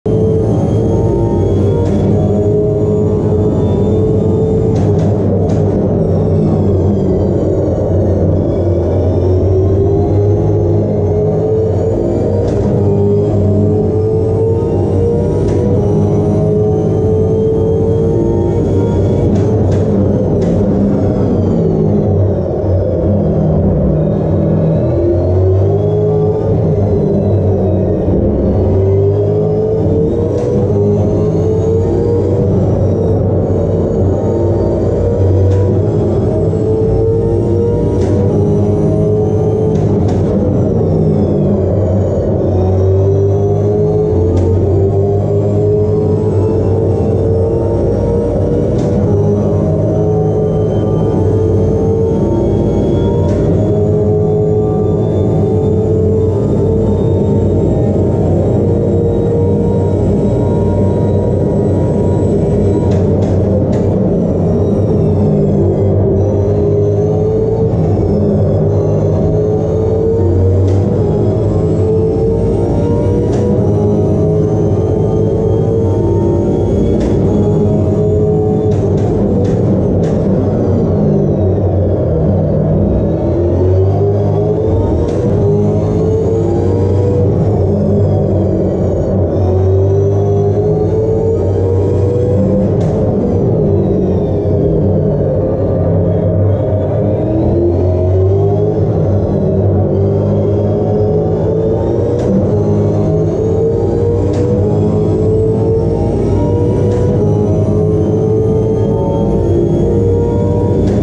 Melbourne Grand Prix 2022 - Focus Simulator
🇦🇺 Melbourne Grand Prix 2022 Layout 🇦🇺

Take a ride around Albert Park in our Supercar model! We’re big fans of the changes, bring on the racing! 😍

If you’re racing at the AGP or simply want to see for yourself the new layout, please get in touch to arrange your session.

Full video available on our Facebook & YouTube!